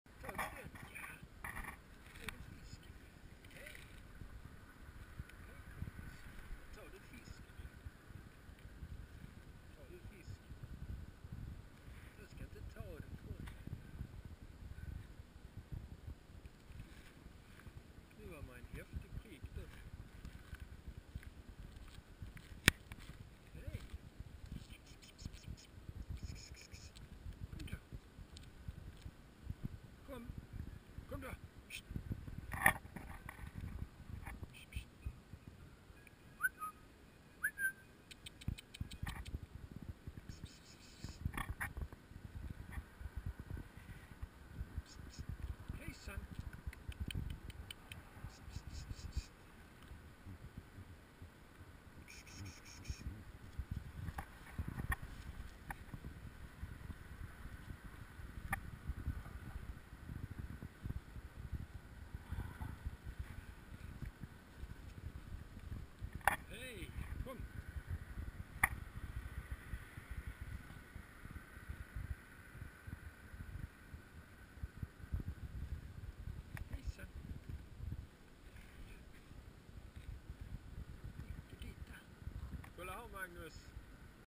American mink in Malmo (Neovison vison) - 1080
American mink is a carnivorous animal trying to steal a fish from me and my friend